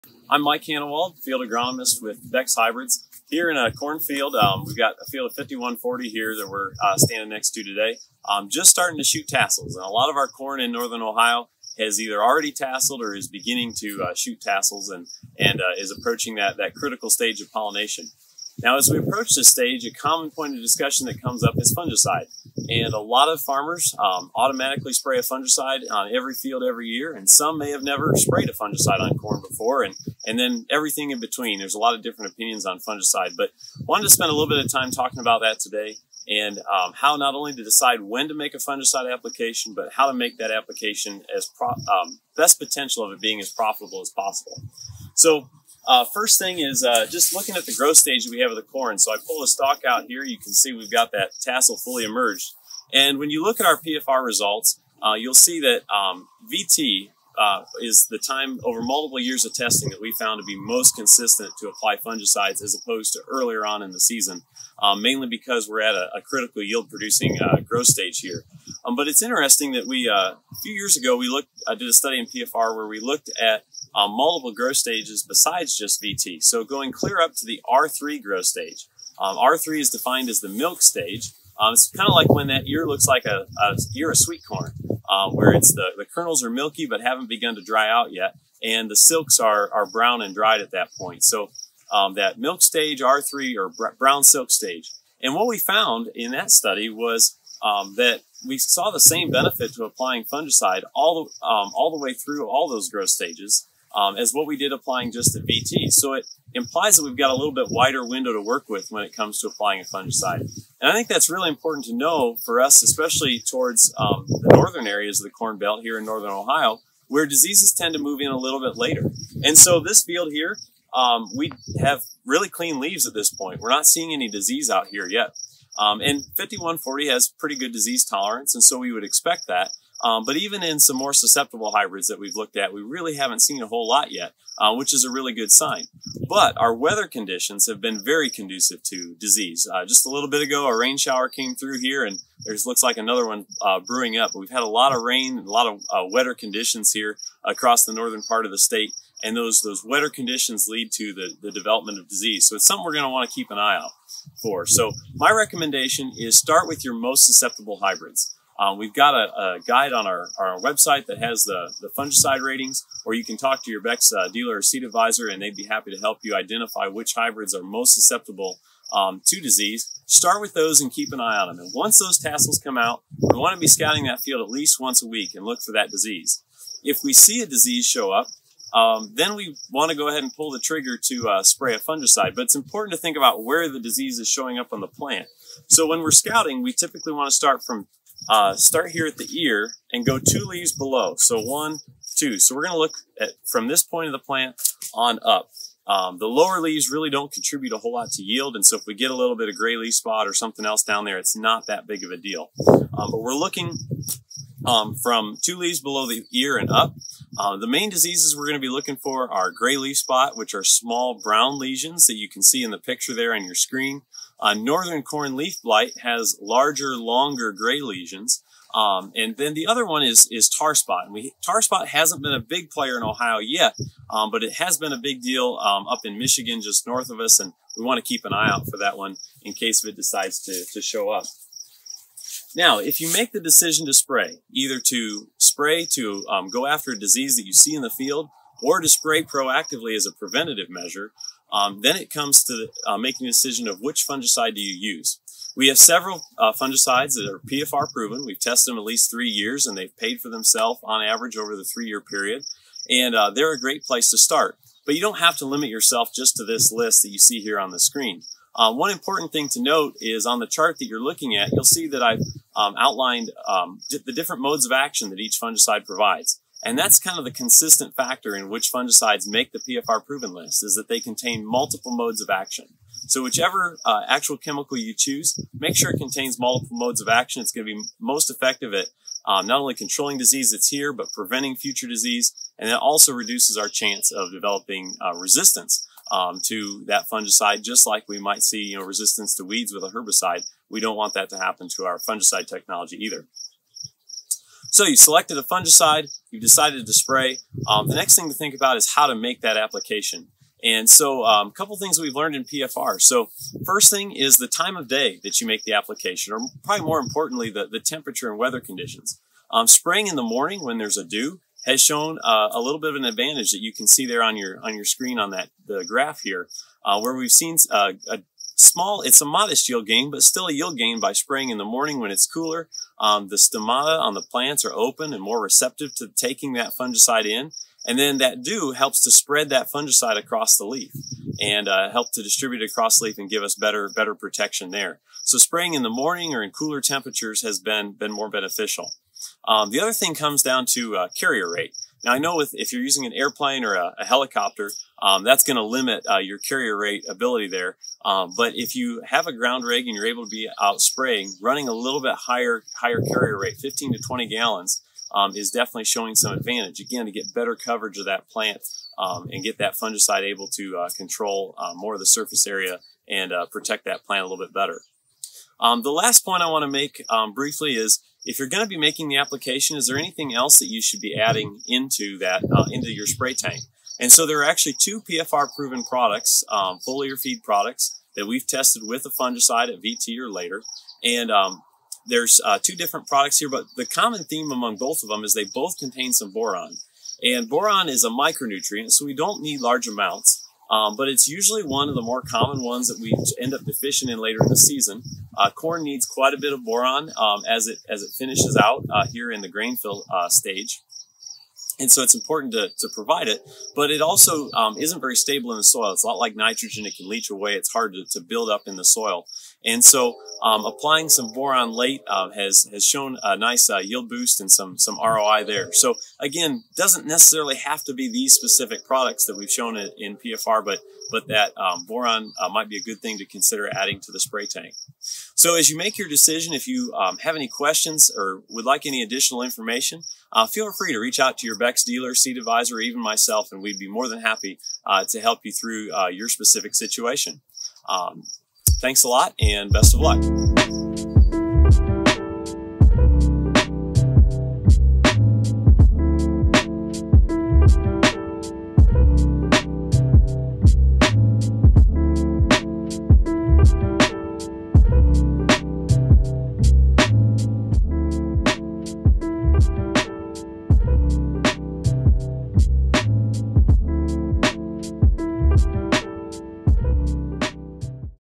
Corn Fungicide | Beck's Agronomy Updates
Check out the new video with Field Agronomist, Mike Hannewald, for important information regarding fungicide decisions on corn.